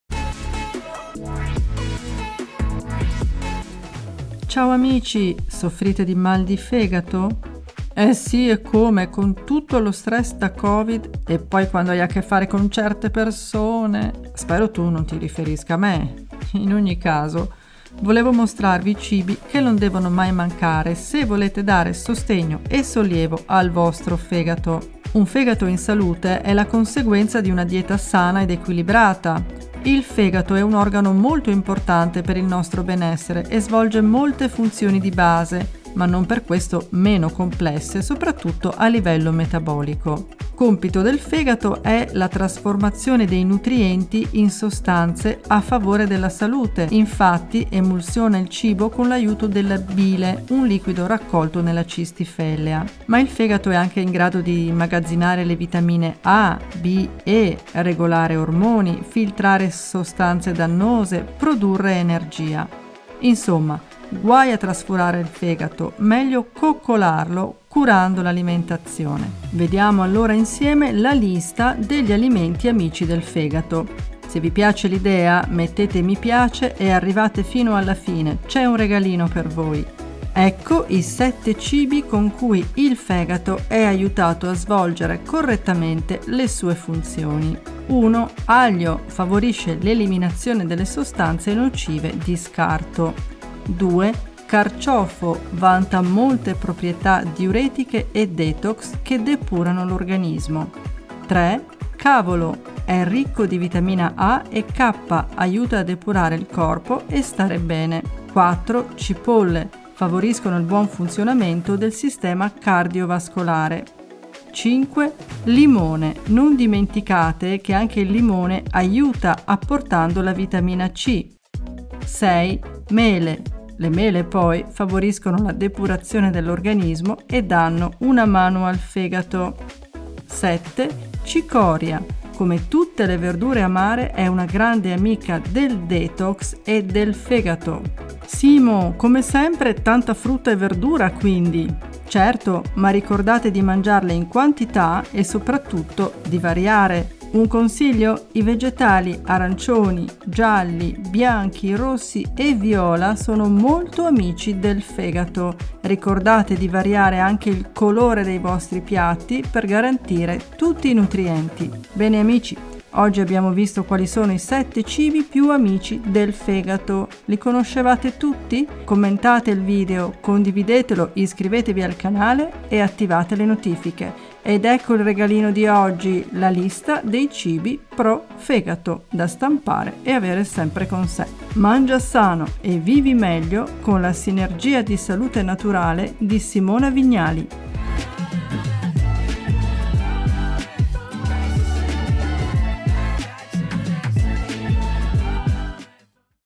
Questi 7 CIBI AMICI del FEGATO sono IDEALI per una DIETA per FEGATO SANO e DEPURATO anti STEATOSI
Fai un detox totale in 7 giorni, è semplice e veloce!

Questa è la soluzione più pratica, gustosa e adatta a tutti per depurare tutto il corpo in modo naturale ❇️

Ciao amici! Per depurare il fegato, mantenerlo in salute e proteggerlo da fegato grasso e steatosi epatica, ecco 7 cibi amici del fegato.

Simona Vignali